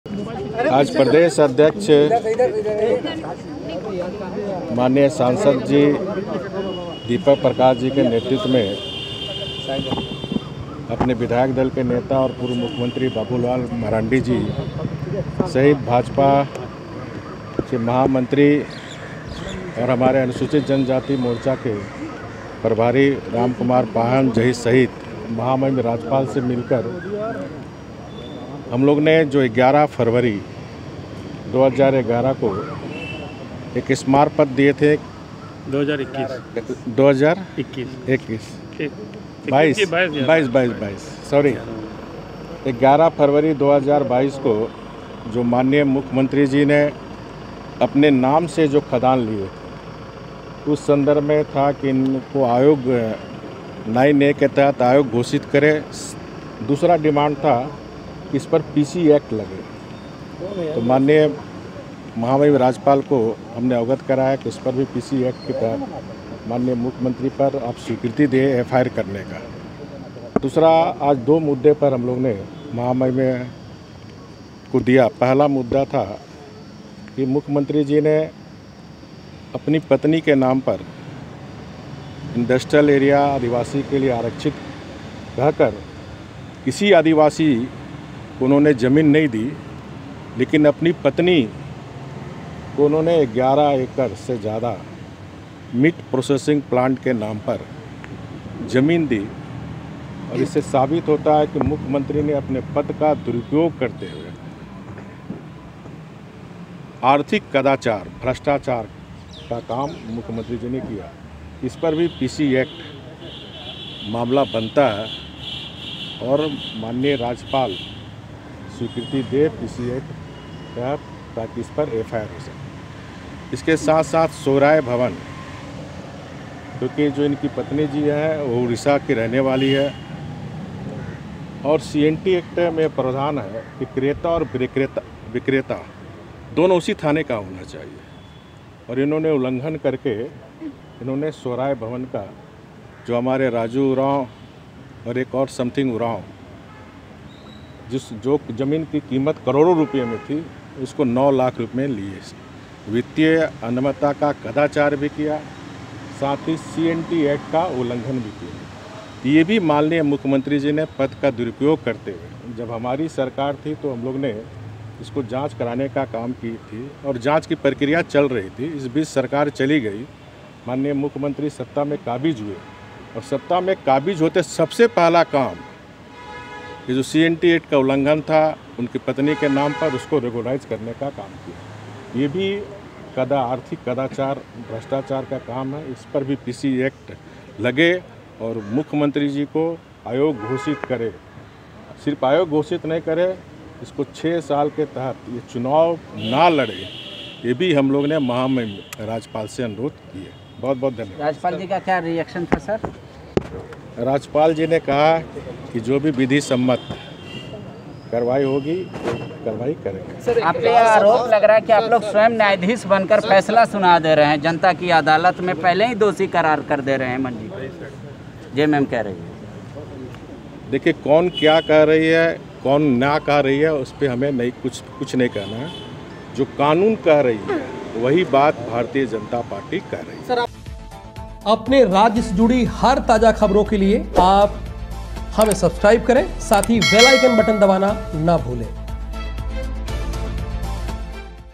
राज्यपाल से मिले रघुवर दास, झारखंड में होने वाला है बड़ा खेला
राज्यपाल से मिले रघुवर_दास, झारखंड में होने वाला है